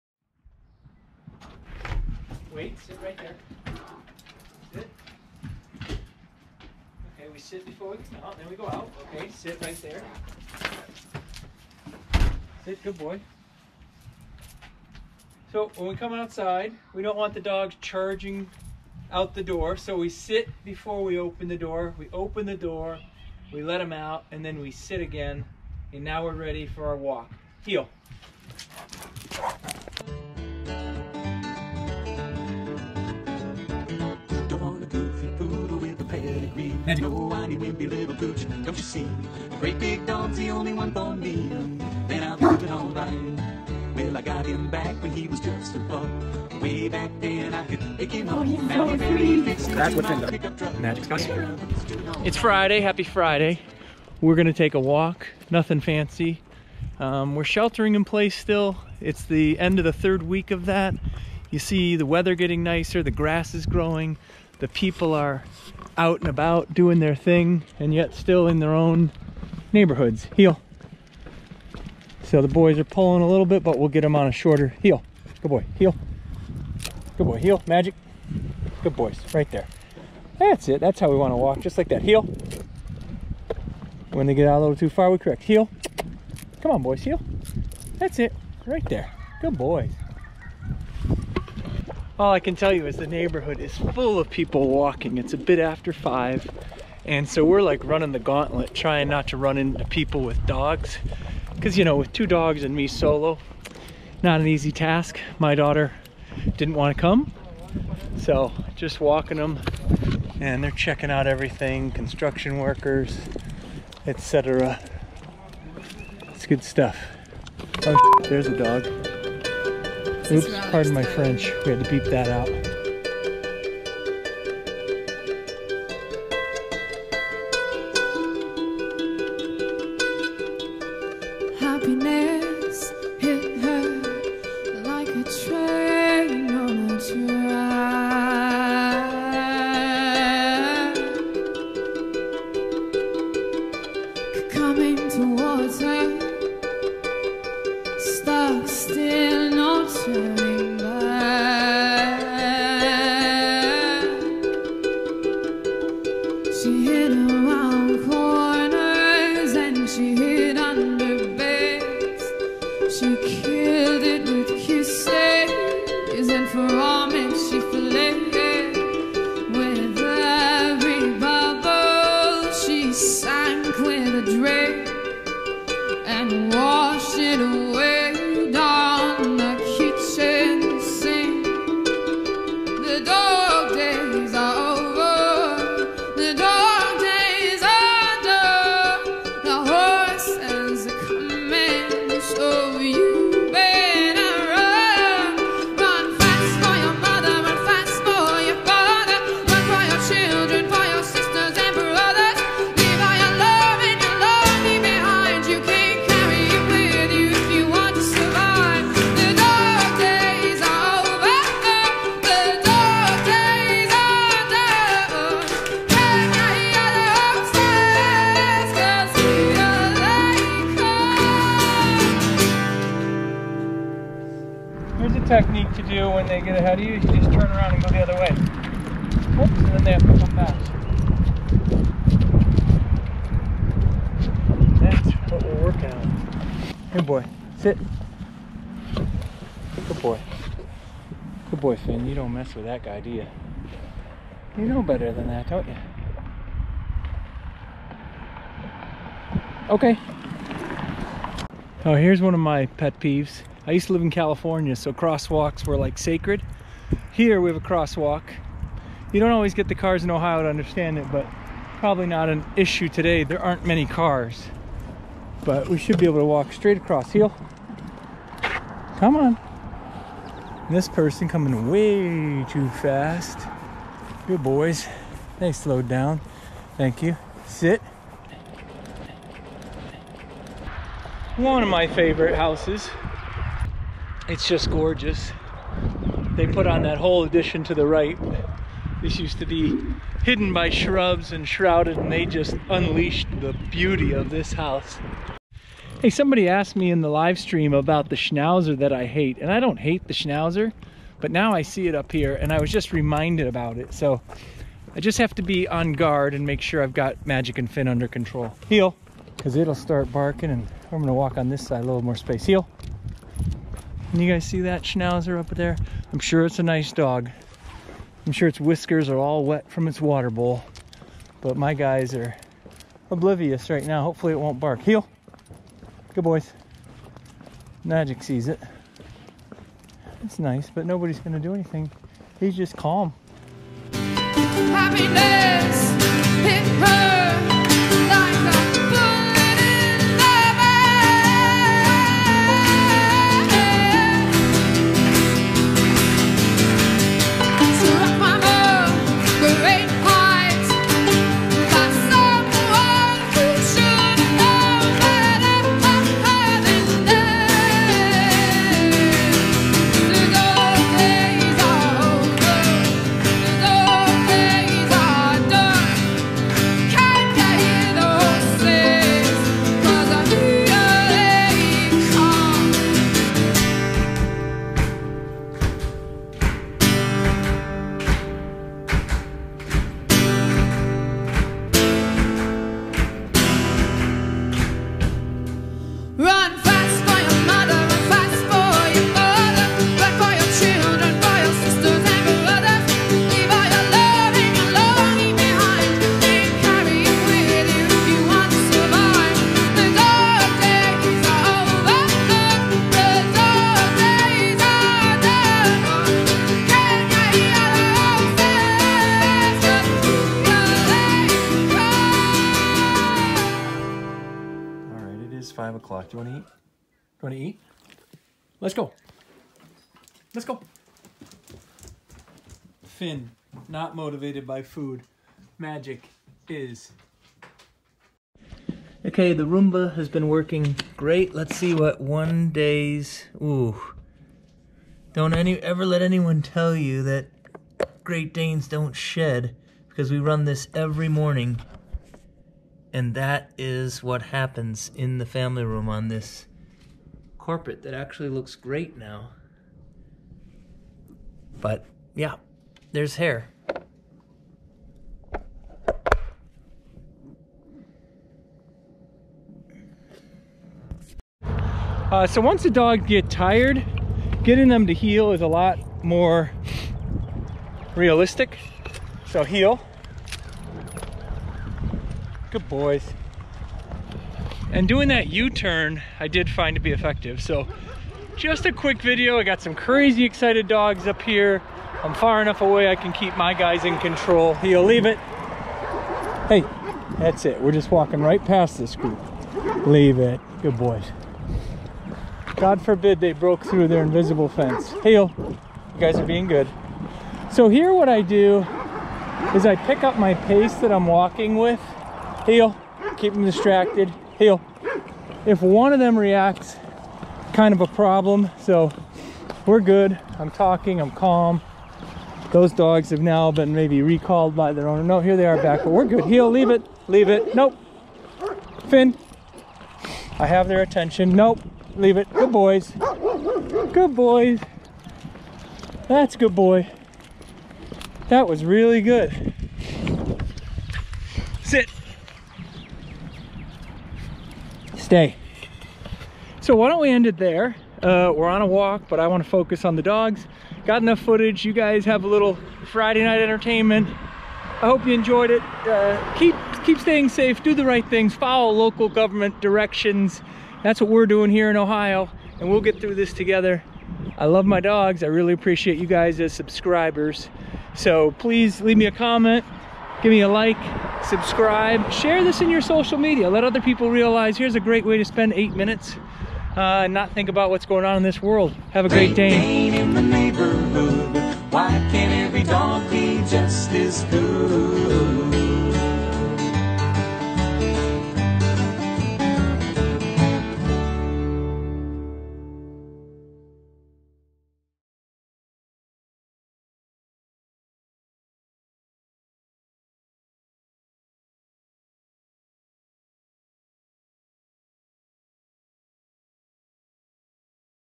Getting 305 lbs of Great Dane to Heel
Great walk with the dogs while still social distancing.

They approached me about reviewing their calming dog beds.
 GreatDanes15